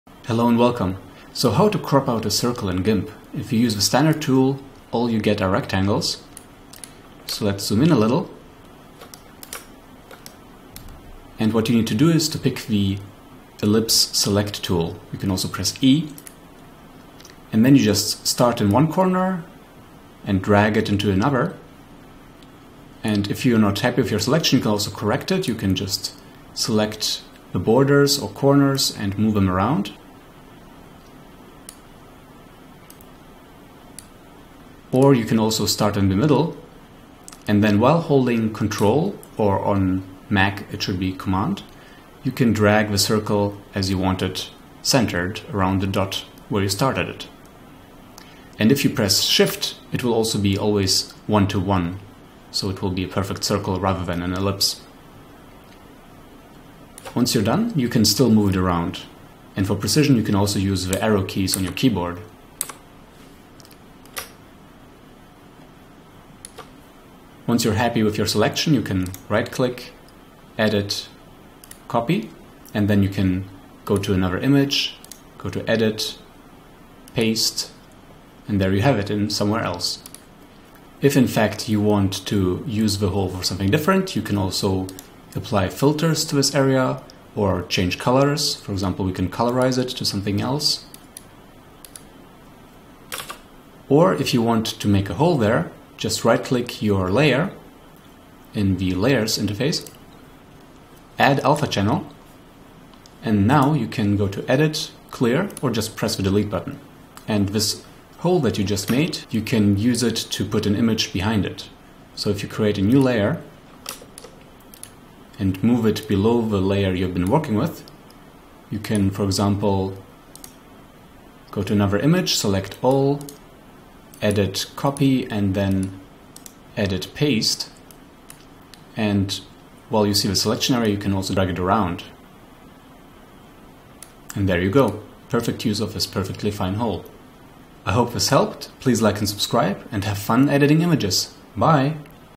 Use GIMP to Crop Circle in Image
Cropping or cutting out a round shape for a circle or sphere in GIMP is easy. Just use the Ellipse Select tool, use CTRL and/or SHIFT (CMD and/or SHIFT on Mac OS X) to support your selection and correct it by zooming in/out and moving corners/edges of the selection. Then you can cut it out, delete it (after adding transparency) or do other things like applying filters and colorization modifications!
This answers the age old question: How to crop a circle out of a picture in GIMP? And no need to use round corners!